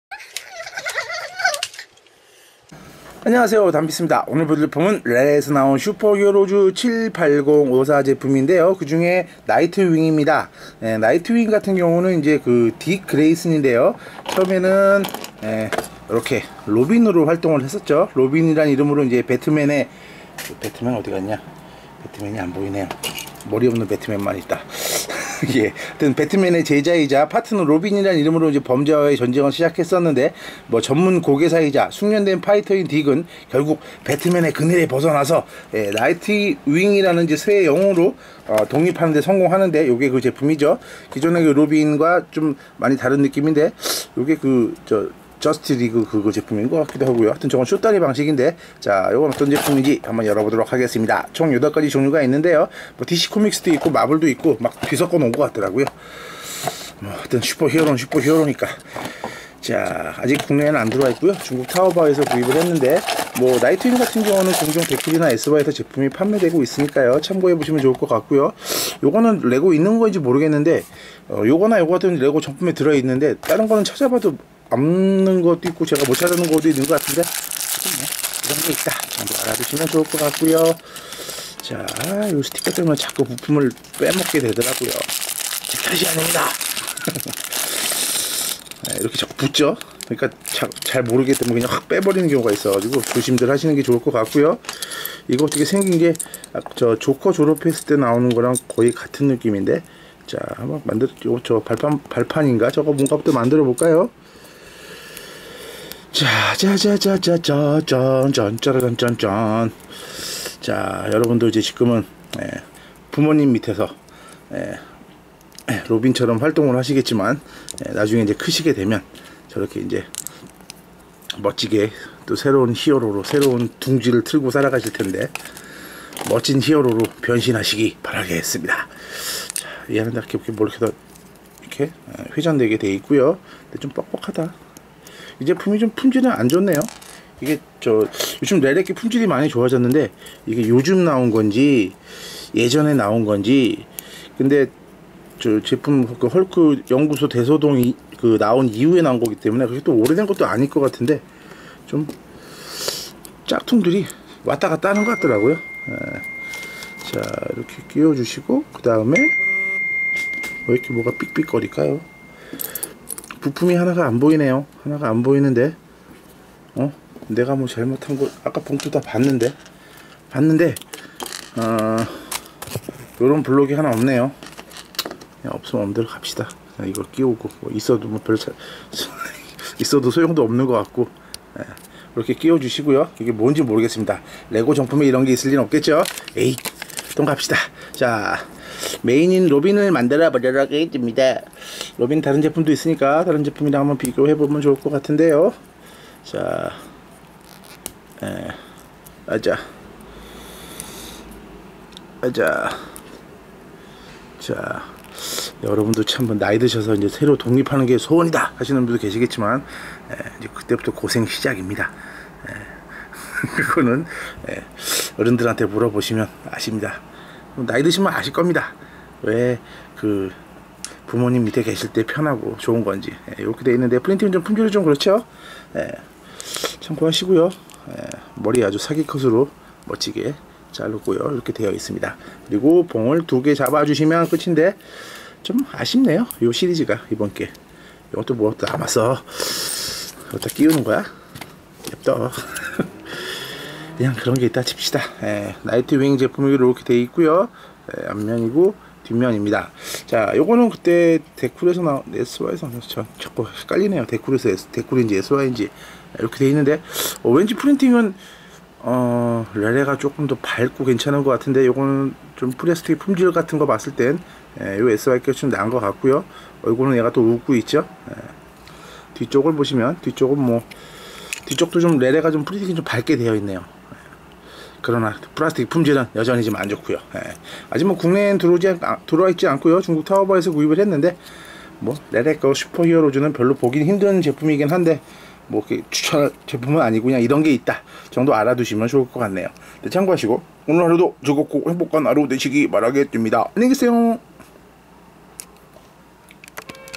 lele 슈퍼히어로즈 나이트윙 레고 짝퉁 미니피규어 리뷰 Lego knockoff Super Heroes nightwing
중국 레고 짝퉁 제조업체인 레레에서 나온 레고 슈퍼히어로즈의 짝퉁 시리즈로 팔콘, 모독, 일렉트로, 배트걸, 태스크 마스터, 플래시, 조커, 나이트윙 등의 8종류의 dc 코믹스와 마블 미니 피규어와 악세사리 모음이 하나씩 들어있는 제품입니다.
그러고보니 레레에서는 슈퍼히어로 시리즈가 별로 나오지 않는데, 예전에 헐크버스터이후에 처음인듯 합니다.

그중에 나이트윙 nightwing의 딕 그레이슨은 배트맨의 제자이자 파트너 로빈이라는 이름으로 범죄와의 전쟁을 시작했어요. 전문 곡예사이자 숙련된 파이터인 딕은 결국 배트맨의 그늘을 벗어나 나이트윙이라는 이름의 새 영웅으로 독립하는 데 성공한답니다.

제품명 - lele super heroes 78054 night wing
가격 -
블럭수 pcs -
판매처 -